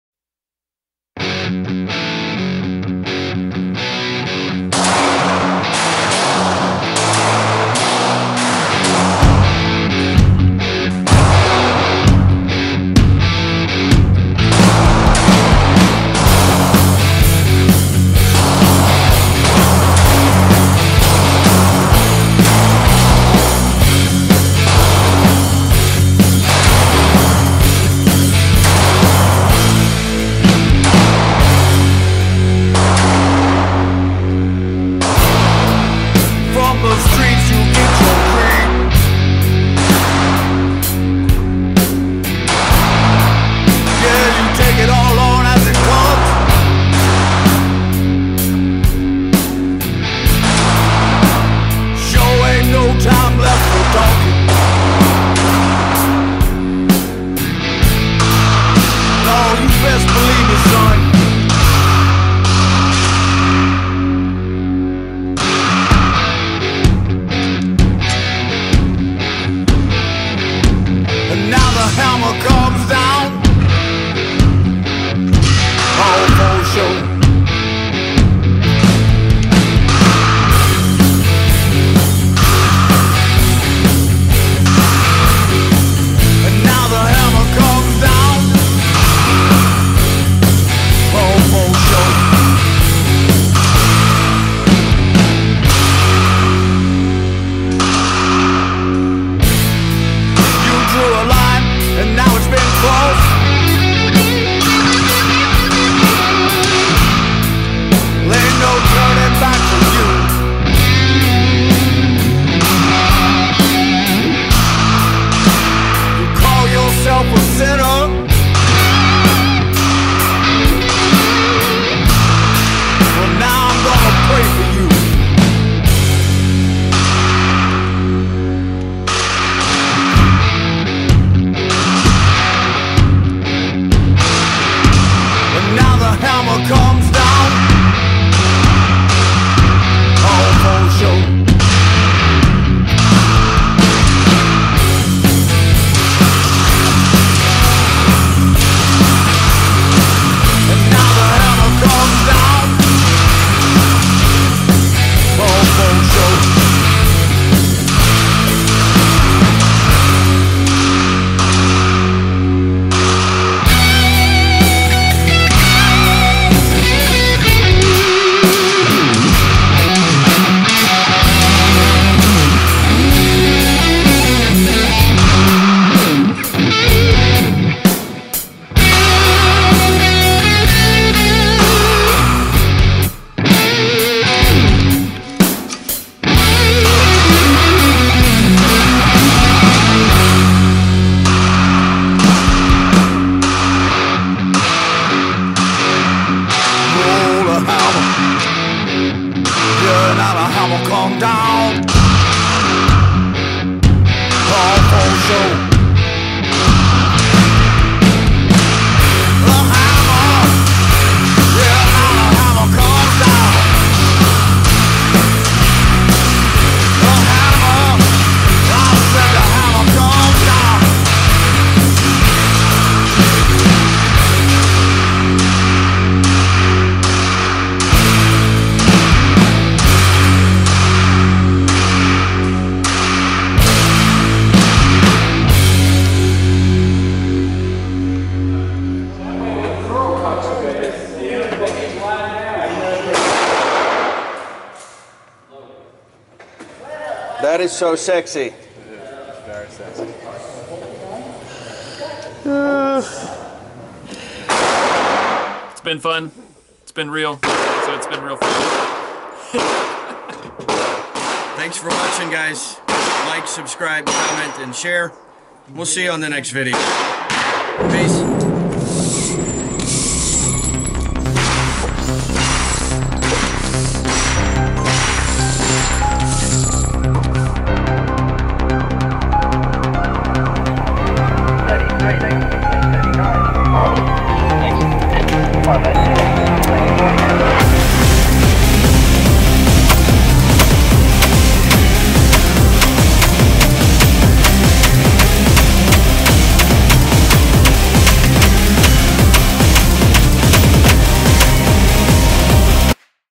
Eagle One Range Time 3-14-15
A little range time with the wife and some friends